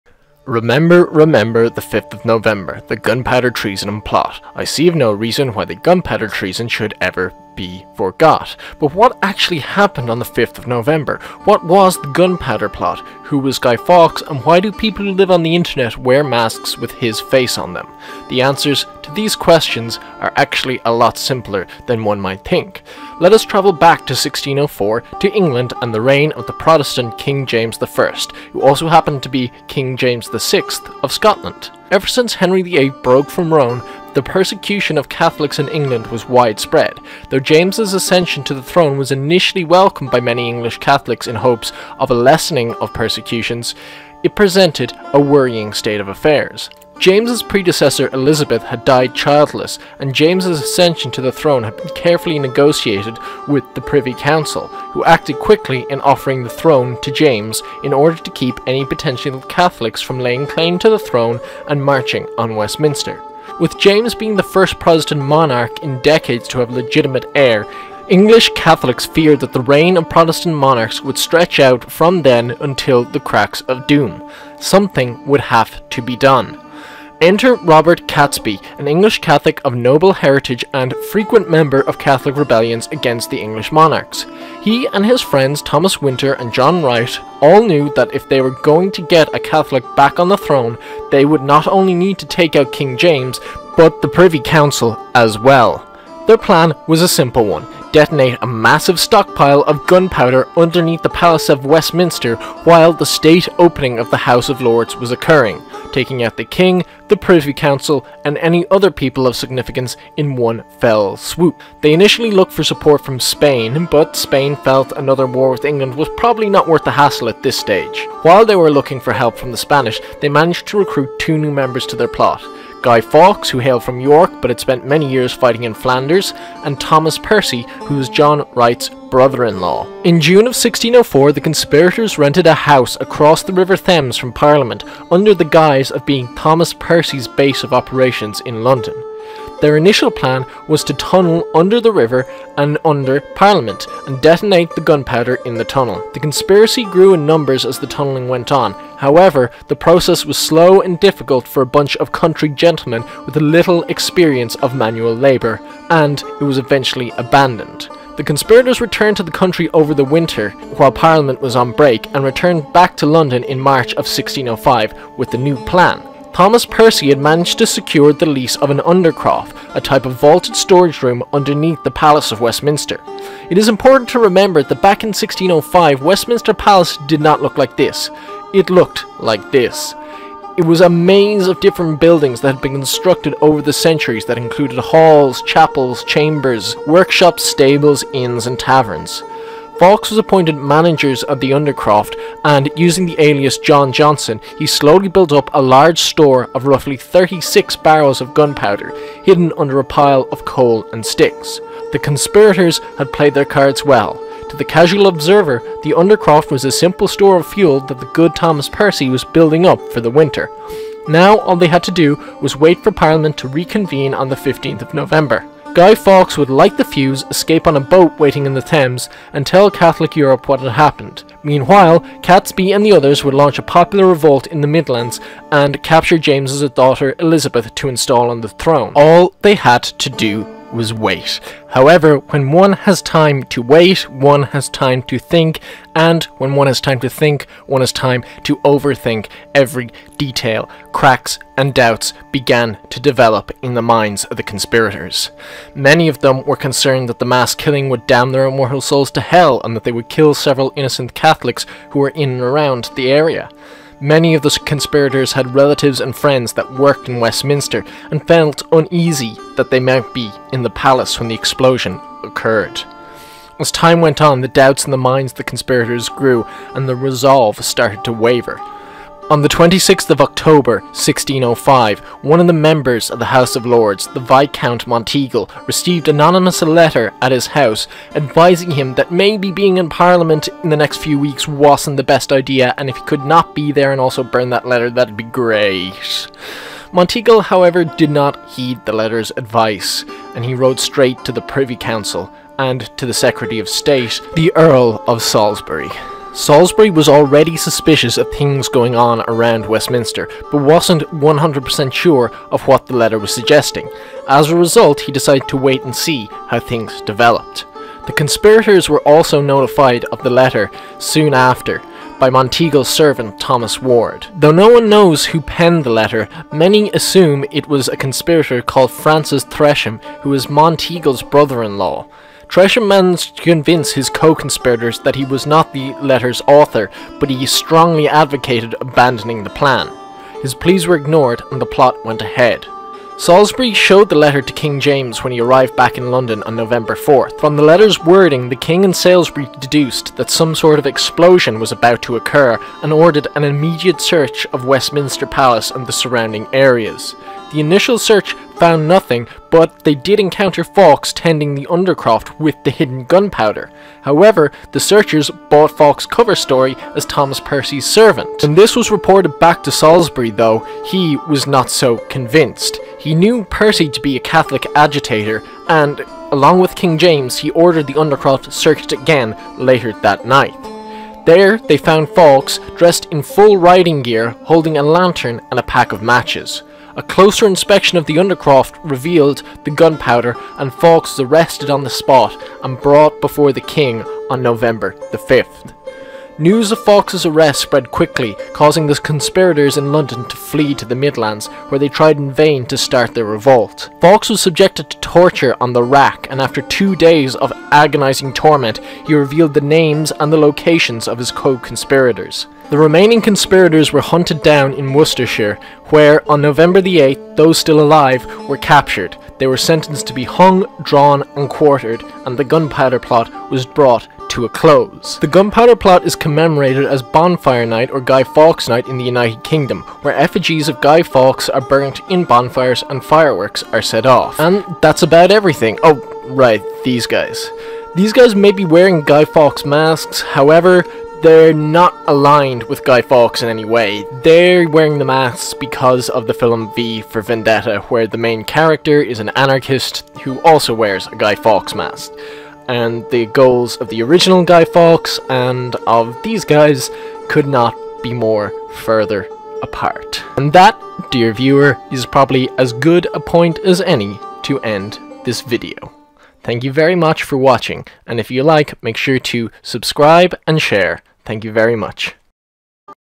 What was the Gunpowder Plot?
"Remember Remember the 5th of November" is pretty well known as are vague detail about the Gunpowder Plot. However a lot more went on and the story is a interesting one that nearly came off successfully.